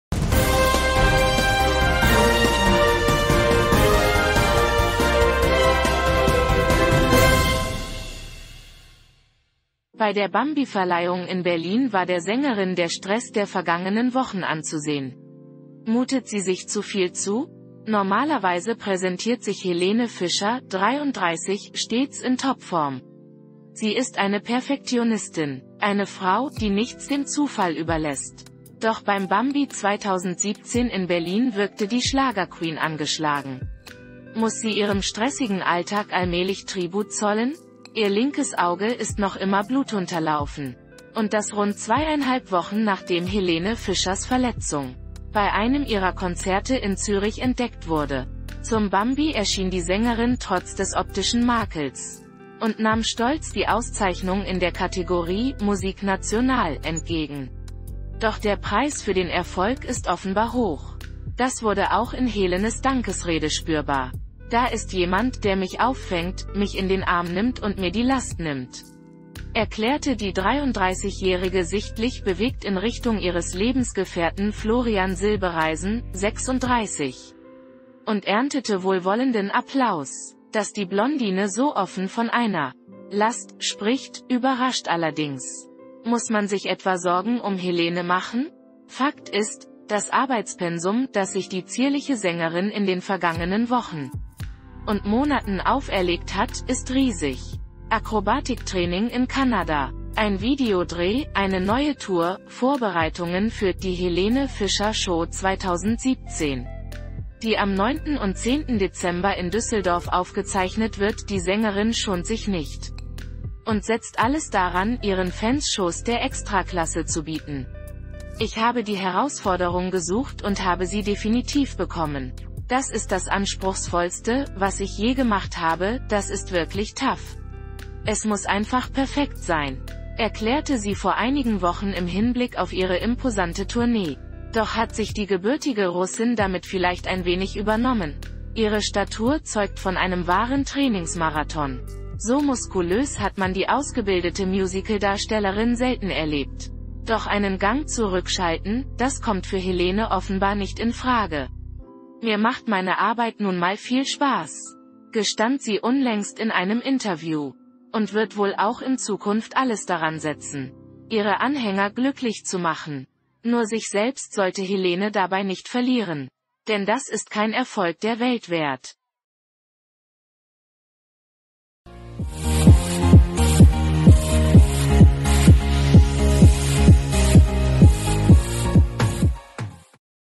Helene Fischer: Warum passieren ihr immer wieder schlimme dinge
Bei der Bambi-Verleihung in Berlin war der Sängerin der Stress der vergangenen Wochen anzusehen.
Mutet sie sich zu viel zu?
Normalerweise präsentiert sich Helene Fischer (33) stets in Topform.
Sie ist eine Perfektionistin. Eine Frau, die nichts dem Zufall überlässt.